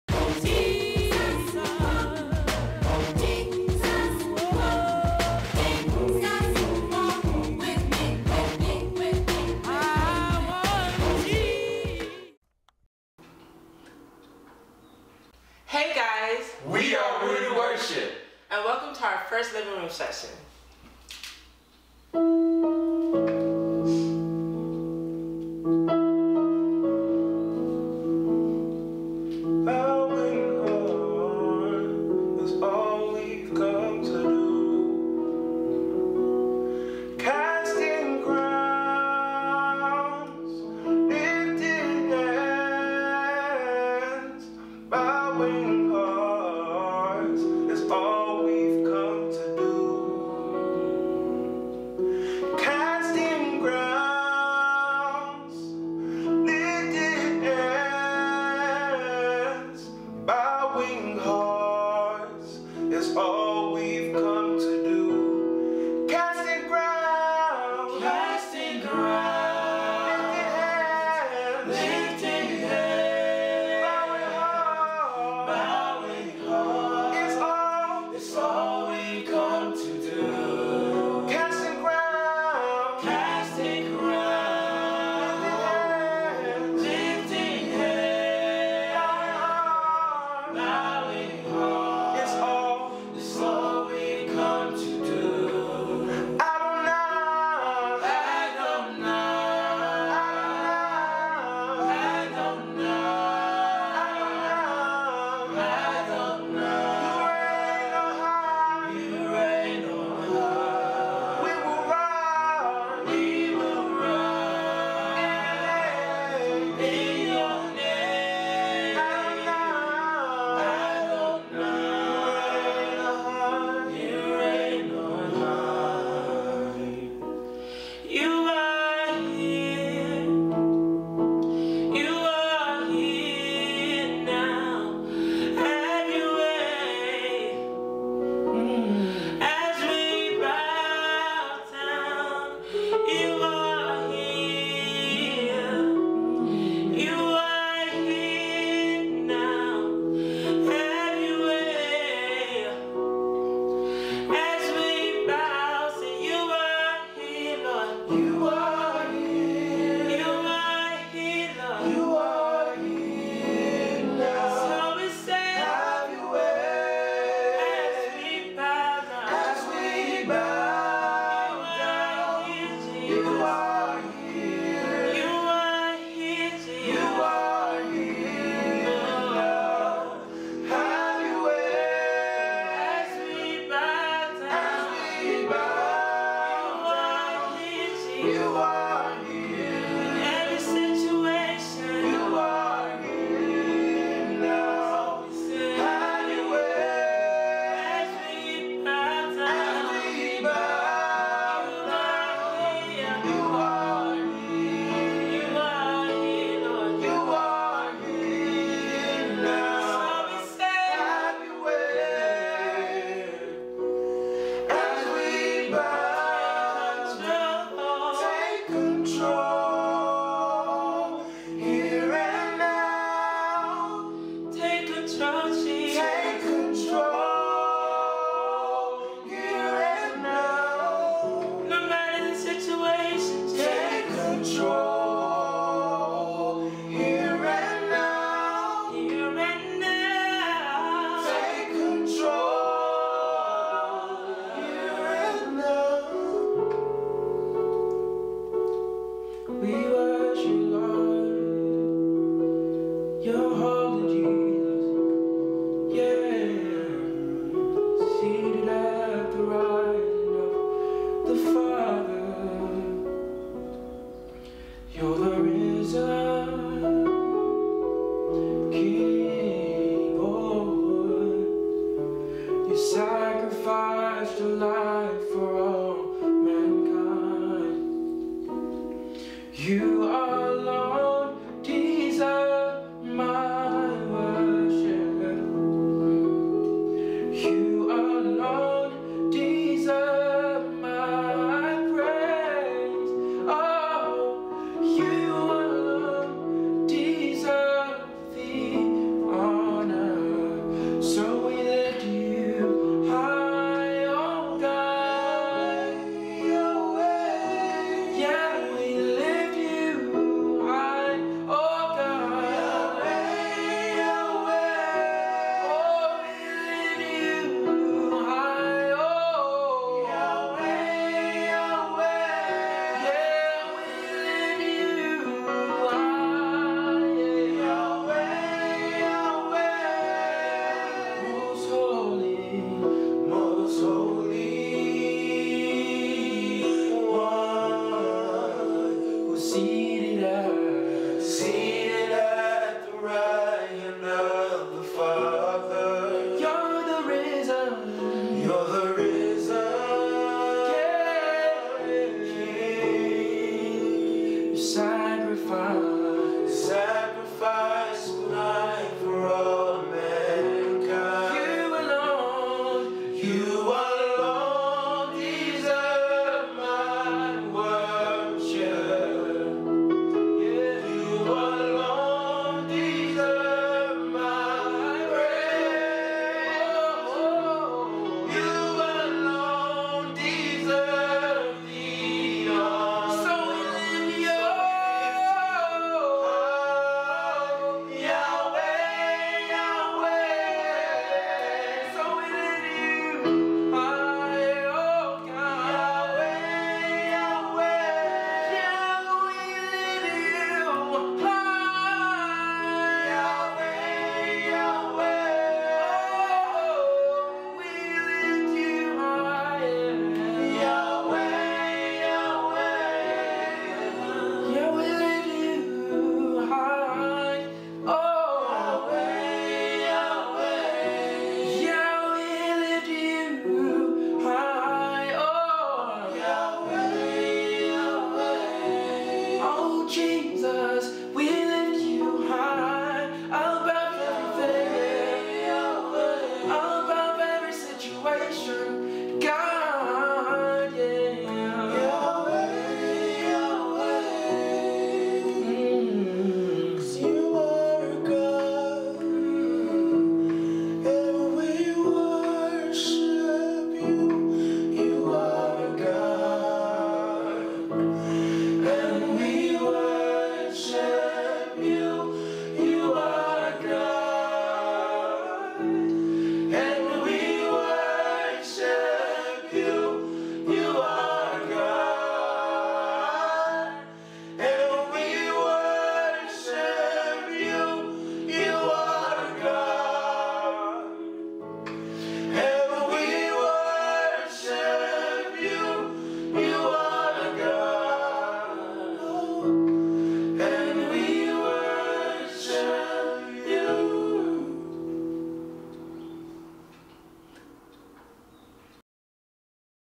TOR Living Room Session Episode #1
Redeemed Christian Church of God, Tree of Restoration is a youth powered church with the goal of building strong young men and women of God.

Artists:
Seun (vocals)
Charles (vocals)
Glory (vocals)
Rere (piano)

We're not perfect but we are purposed